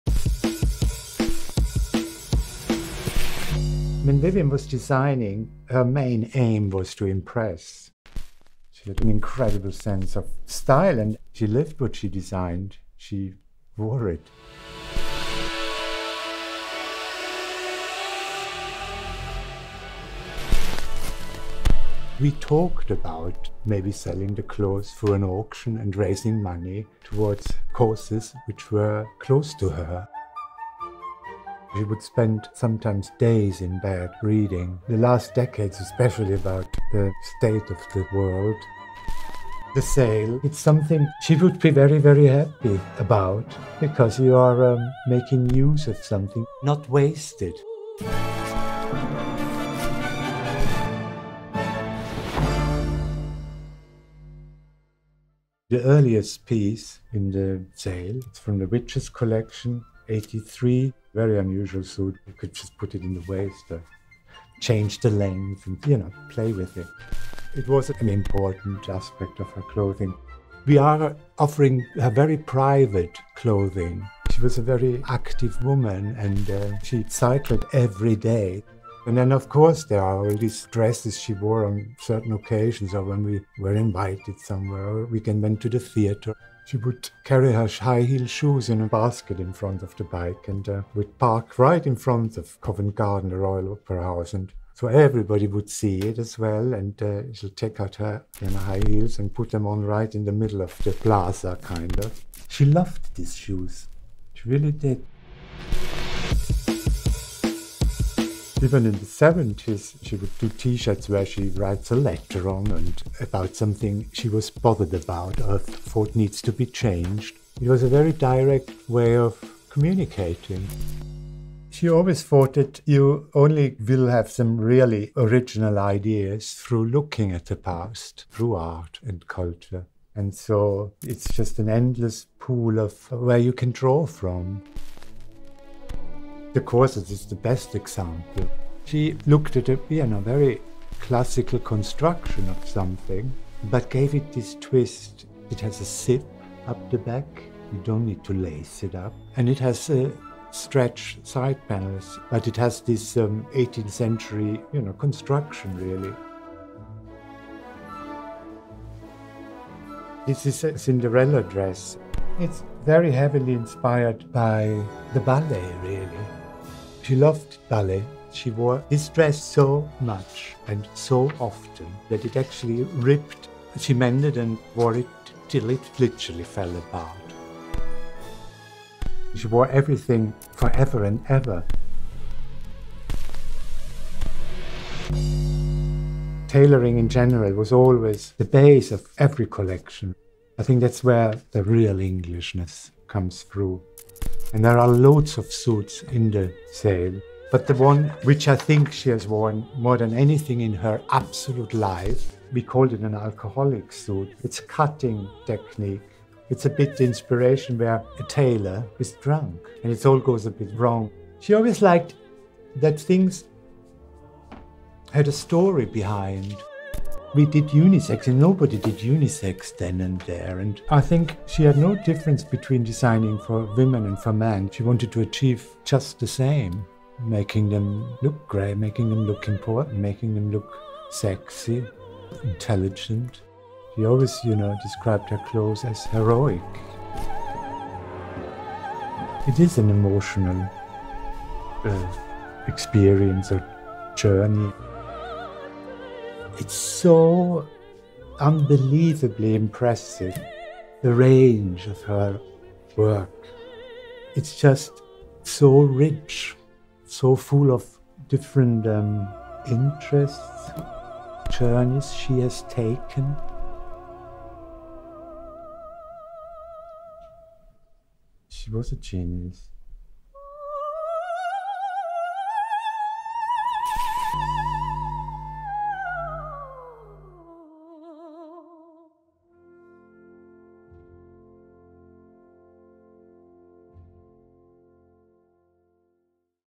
Vivienne Westwood: The Personal Collection | Christie's
Dame Vivienne Westwood is recognised globally as one of the most influential designers of modern times, establishing one of the world’s leading fashion brands. Andreas Kronthaler, Vivienne’s husband and Creative Director of Vivienne Westwood®, has selected iconic looks from Vivienne's wardrobe to be presented across two auctions: a live sale taking place in London on 25 June with an online auction taking place alongside from 14 to 28 June 2024.

The sale will raise funds for the causes Vivienne supported throughout her life, with proceeds going to support The Vivienne Foundation, Amnesty International, Médecins Sans Frontières and Greenpeace.

Vivienne Westwood: The Personal Collection comprises the personal wardrobe of the late fashion designer and includes some of the most important iconic looks that she created and wore during the last four decades of her life. The wardrobe, which only contains garments, shoes and jewellery by Dame Vivienne Westwood’s eponymously named fashion house, is recognised as historically and culturally significant — the earliest piece coming from the Witches collection for autumn/winter 1983/84. The live auction (Part I) on 25 June will also include THE BIG PICTURE – Vivienne’s Playing Cards: Collect the cards. Connect the cards. In 2017, Dame Vivienne designed a pack of playing cards as ‘A Strategy 2 Save the World’ — a blueprint for the preservation of the planet, infused with her rallying cry, ‘Collect the cards. Connect the cards.’ The prints poignantly shed light on pressing issues facing humankind, such as climate change, social inequality and human rights — concerns that remained at the forefront of Dame Vivienne’s mind. Shortly before she died, she selected ten of her most powerful graphics to be enlarged and set about signing the precious large-scale ‘cards’ on which these designs would be printed posthumously. A project by The Vivienne Foundation, proceeds from the sale of the portfolio will benefit Greenpeace, according to her wishes.

Dame Vivienne Westwood is recognised as one of the most important British fashion designers of all time and, by many, as the most iconic fashion designer of the last quarter of the 20th century. Westwood used her influence to address some of the gravest issues facing humanity today. She established The Vivienne Foundation to partner with NGOs with similar aims: ‘to create a better society and halt climate change’.